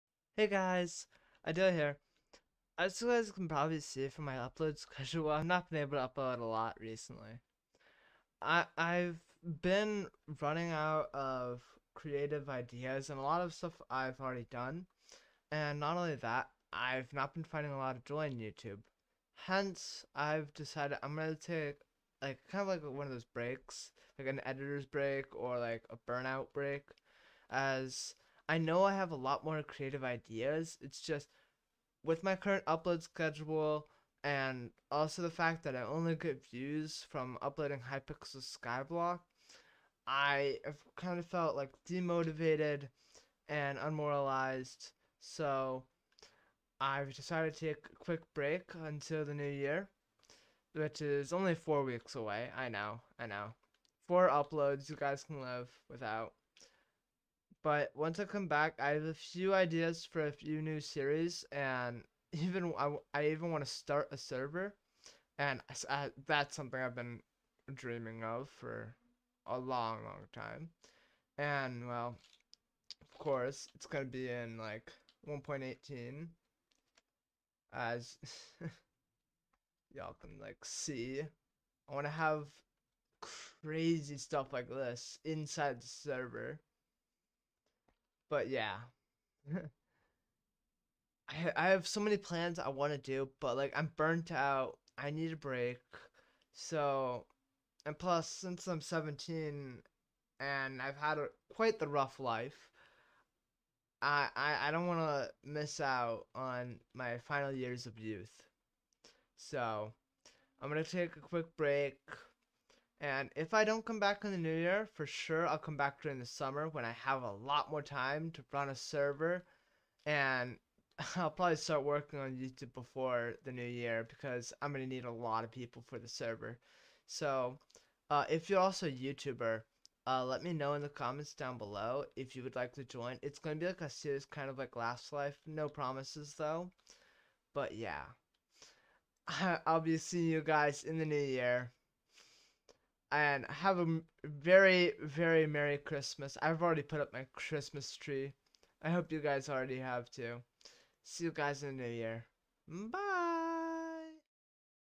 Im Burned out....
Little explanation here
lately ive been trying hard on youtube but with my schedule I cant work on youtube at all until the week end and by that time im demoralized and thus I try but all I can do is make hypixel skyblock content because that makes the views roll in and I wanna make content that makes views come in but I had fun making it. I hope you guys under stand so until the new year or summer Im going to take a break so i can rest and plan two new series.

IF YOUR A YOUTUBER OR KNOW ONE COMMENT I WANNA RECURT SOME FOR THE SERVER I WILL MAKE IN THE SUMMER.